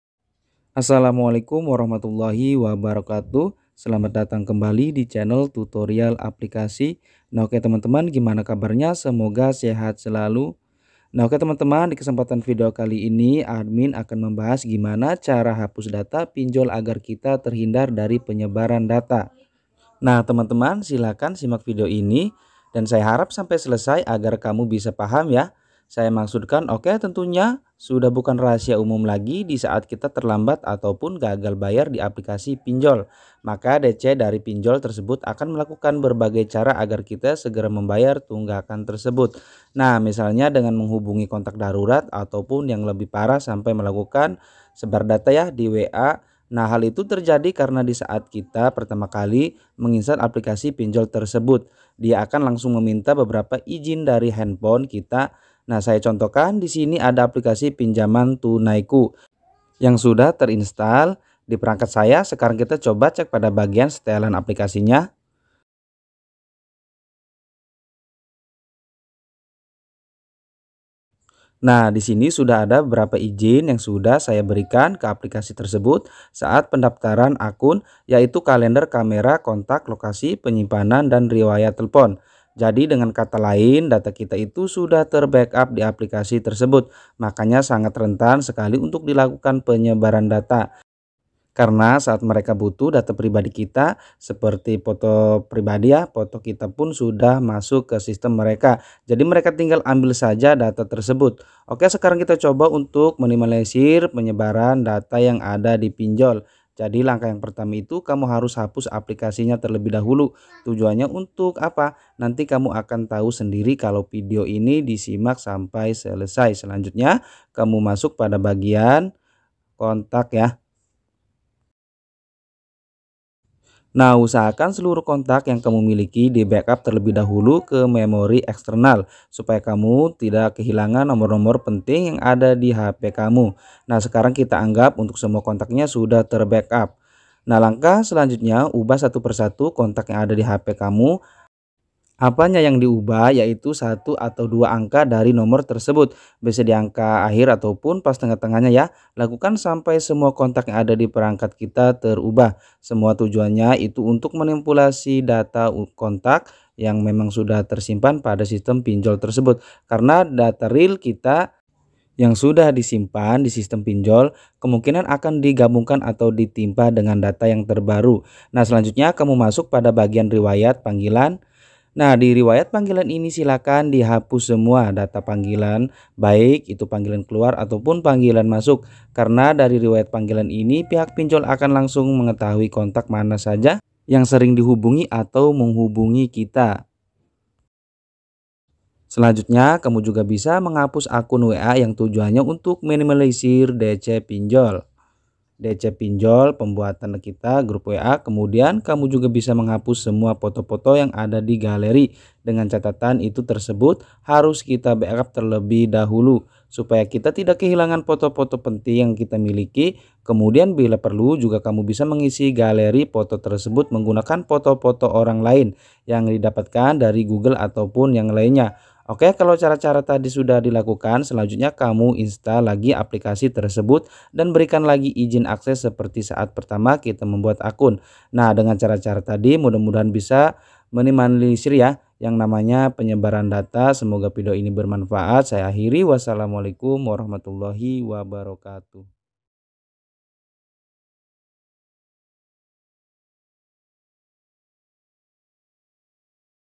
Berhasil !! Cara hapus Data Pinjol 2024 - 100% Galbay Pasti Aman
Bagi teman teman yang lagi galbay sebelum data kita di sebar yang ingin menghapus data pinjol kita di aplikasi  simak video ini sampai selesai..